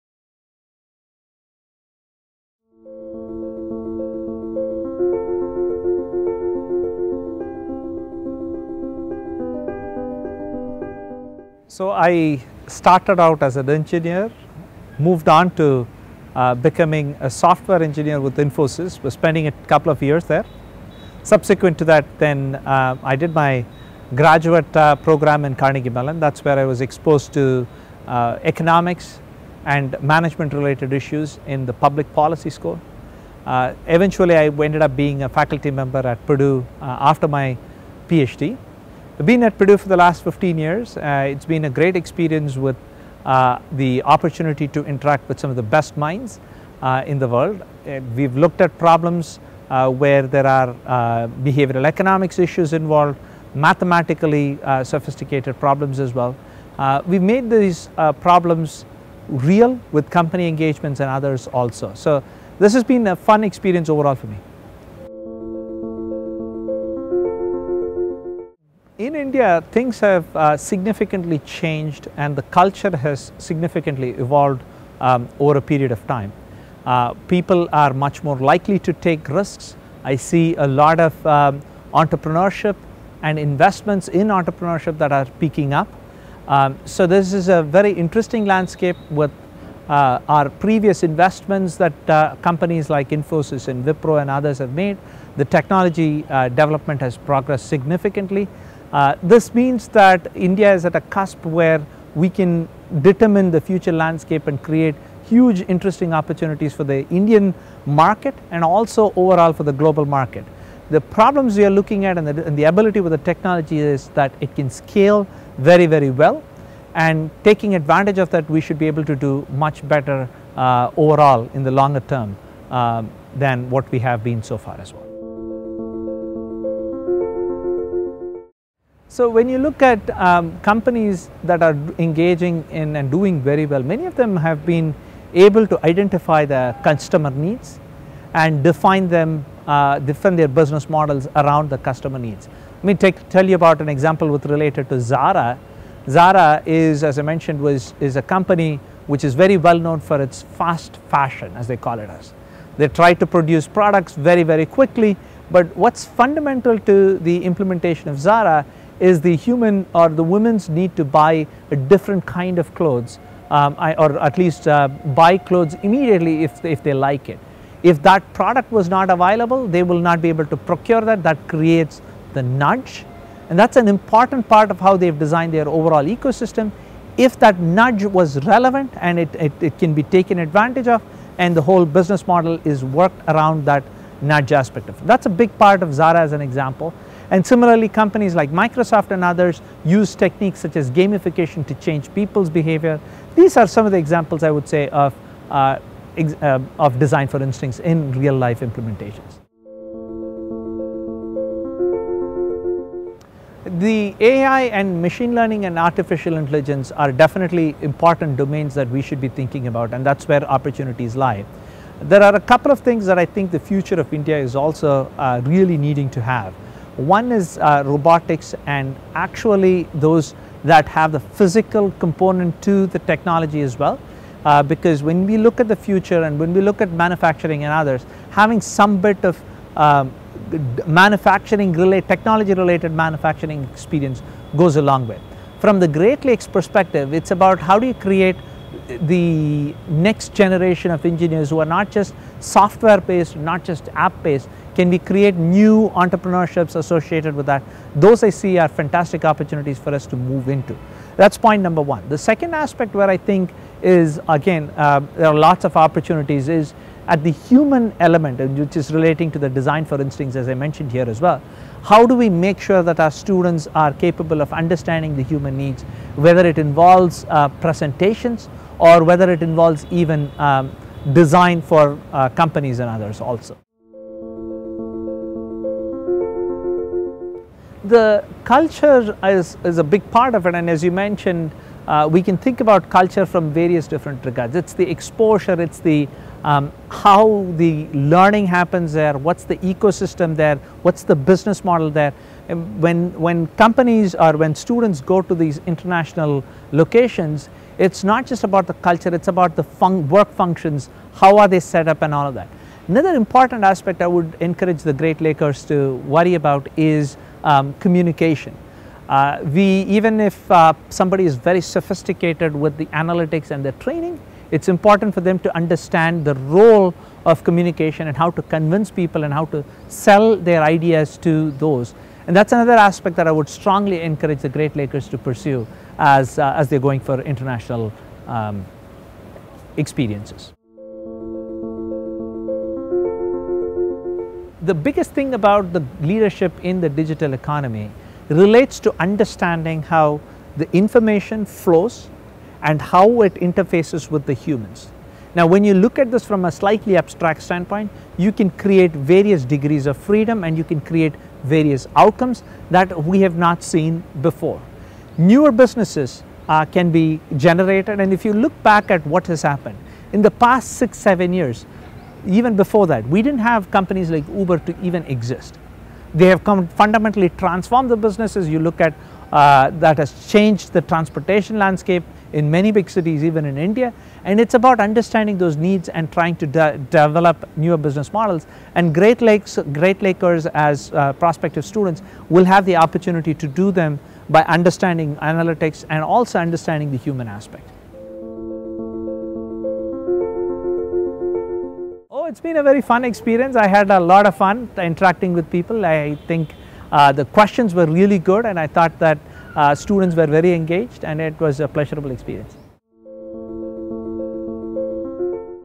In Conversation with Prof. Karthik Kannan, Director - BI & Analytics @Krannert School of Management
Recognized as a thought leader in digital transformations, analysis of digital traces, and strategic foresighting, Prof Kannan is currently the Thomas Howatt Chaired Professor in Management and director for BIAC (Business Information and Analytics Center)  at Purdue's Krannert School of Management. During his visit to Great Lakes, he shared insights on the digital landscape of India and his journey to becoming a globally renowned educator.